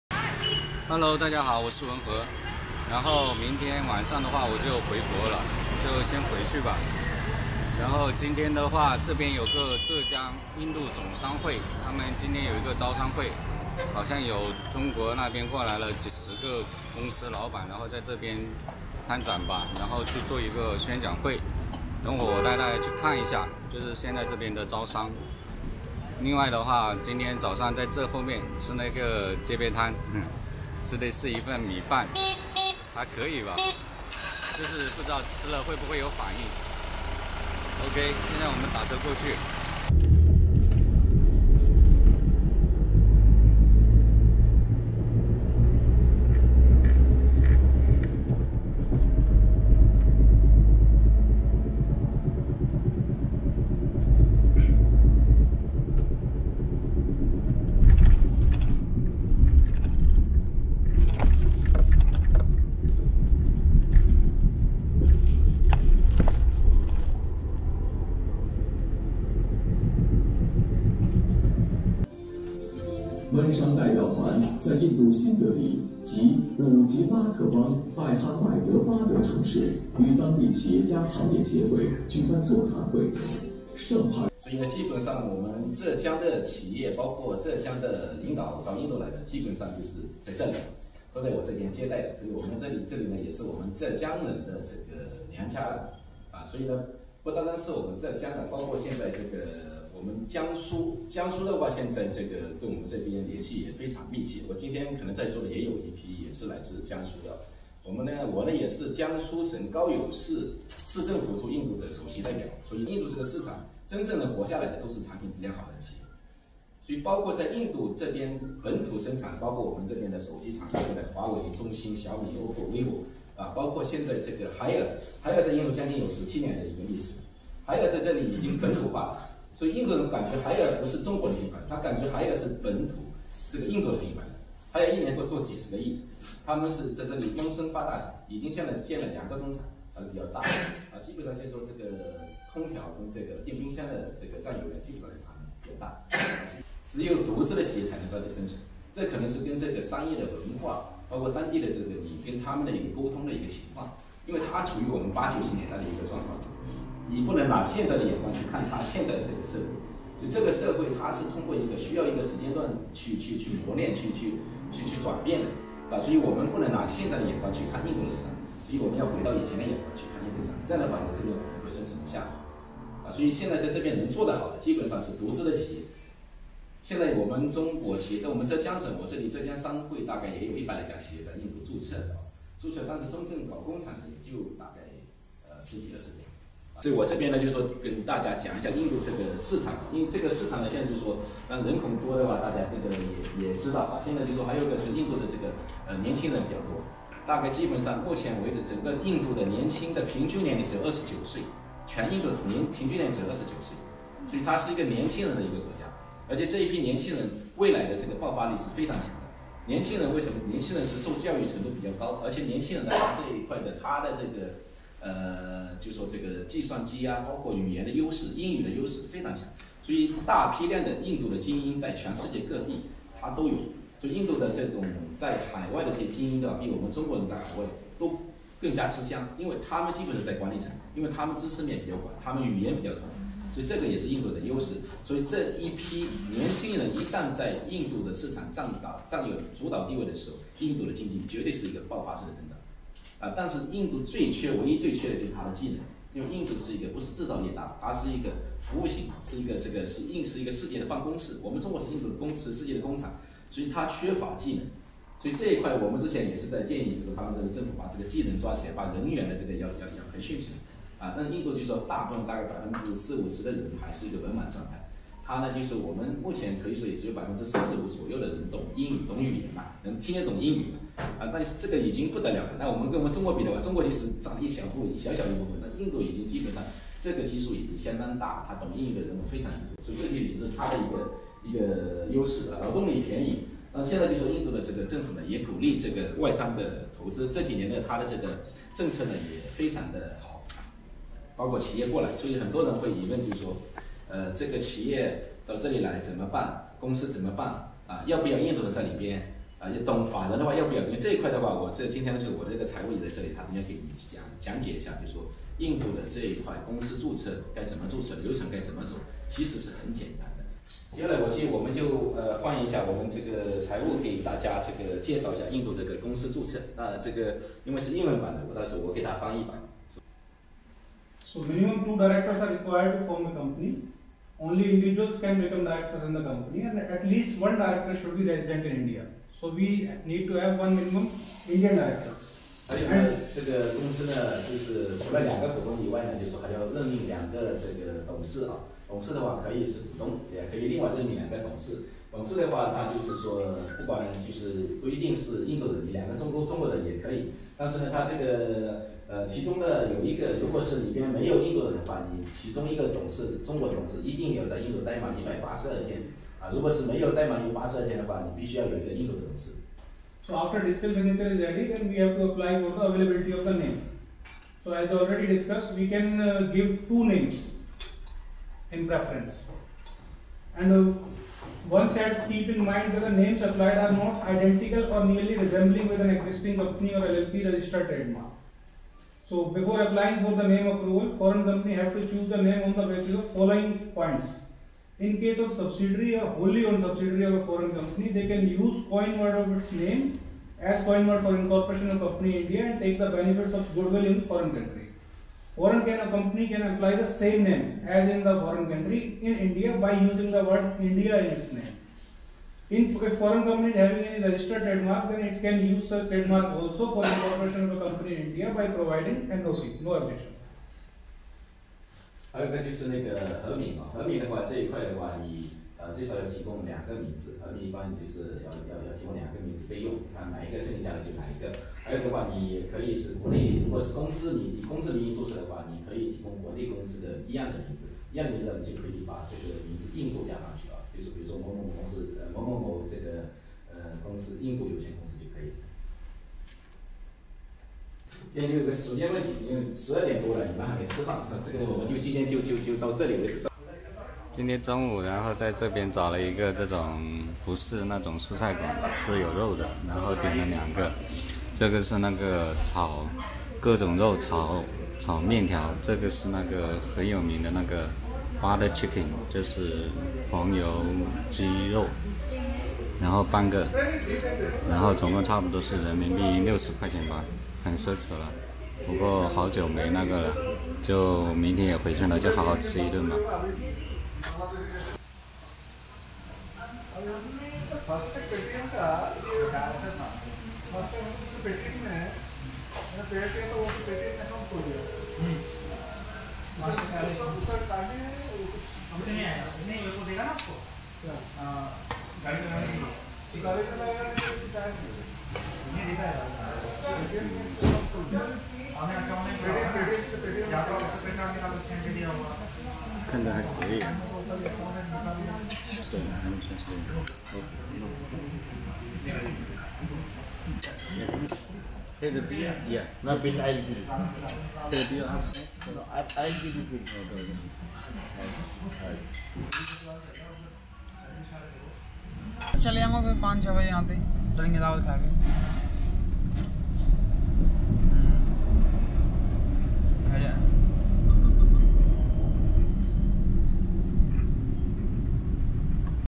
印度浙江总商会会长跟大家讲解印度市场现状及企业如何入驻印度
度浙江总商会会长跟大家讲解印度市场现状及企业如何入驻印度市场。
视频观点仅供大家参考，谢谢！
如果喜欢我的视频，欢迎大家点赞、留言并关注，谢谢！